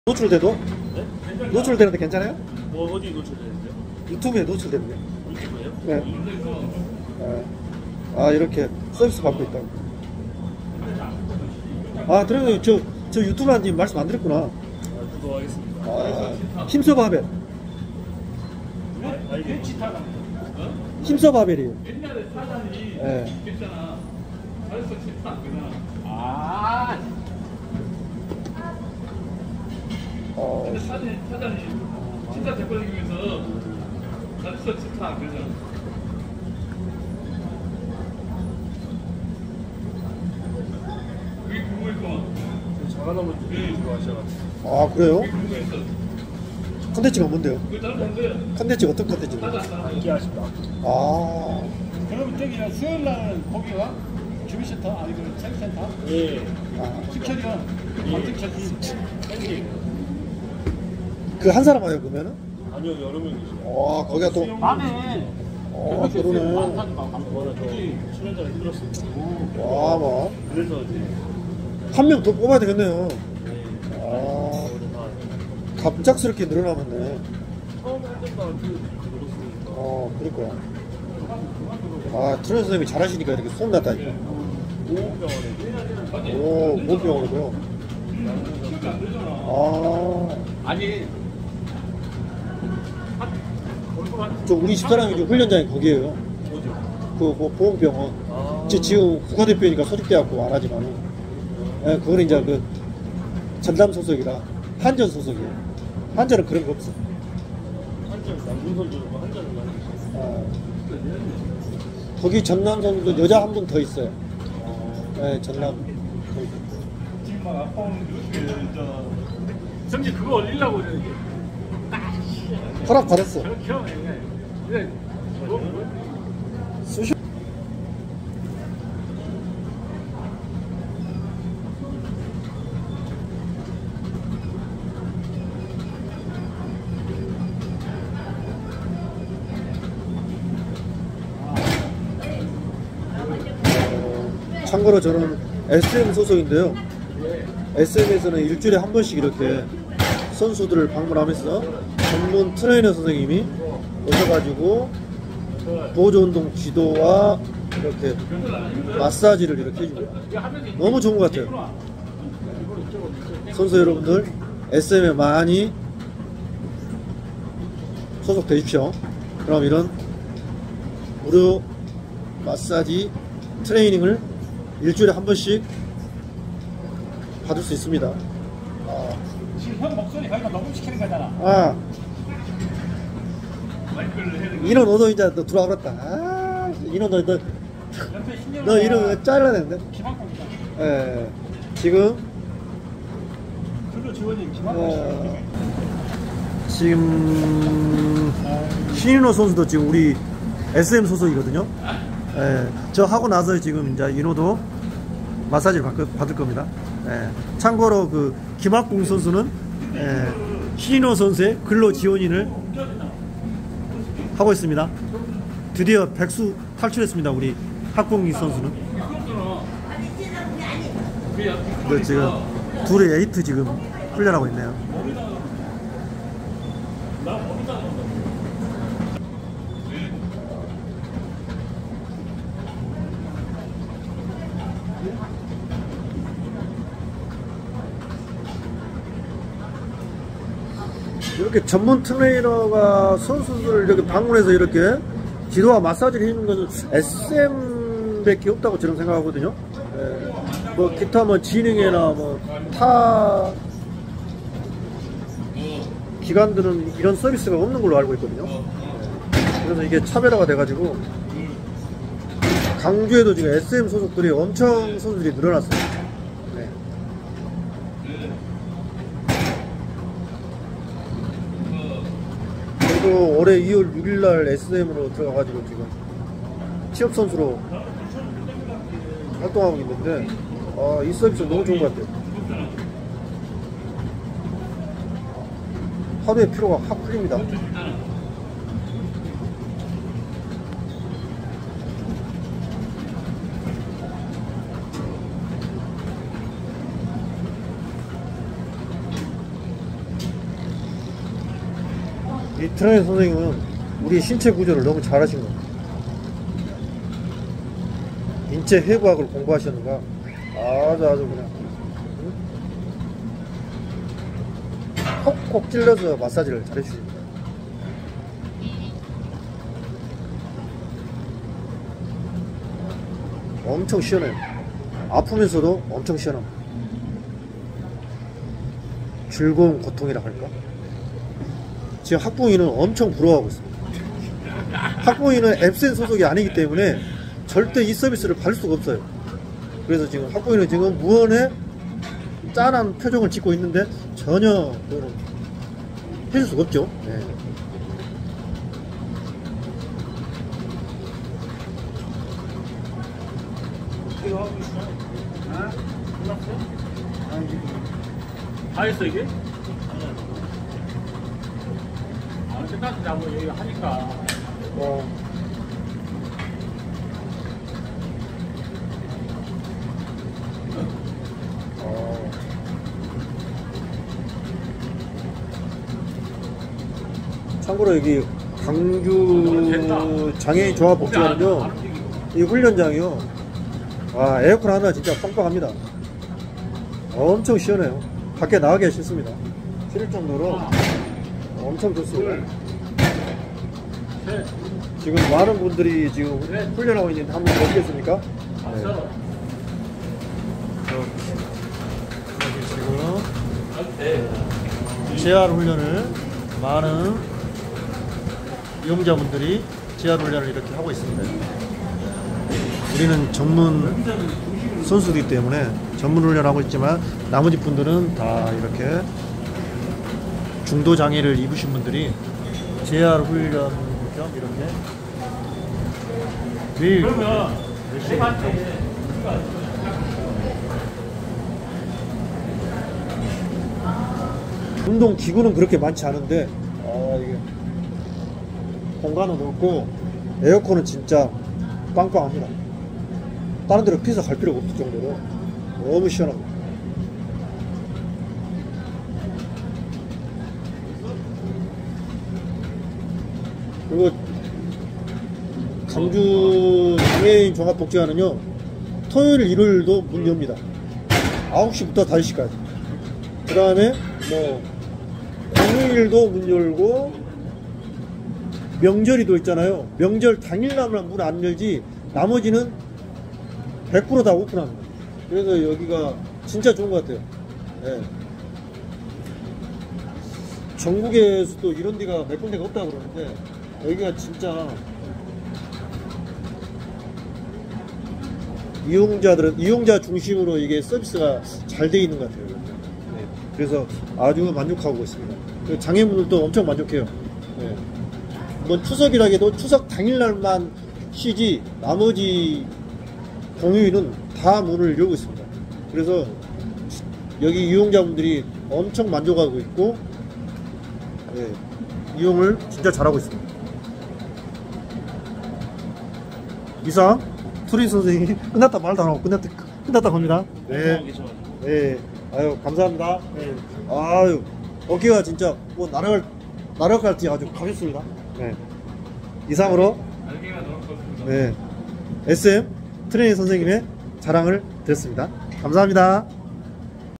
갤럭시아에스엠? 취업선수를 1주에 한번 찾아 트레이너가 마사지와 기초체력관리#장애인역도(지체1급)[56]
김성우트레이너샘께서 매주 오셔서 훈련으로 뭉친 근육통을 풀어주시고 기초체력까지 관리 해주시는데 늘 감사하고 있습니다..
항상 선수들이 있는 곳에 나타내서 애쓰시는데 안전운전하세요..
갤럭시아 에스엠 화이팅!!!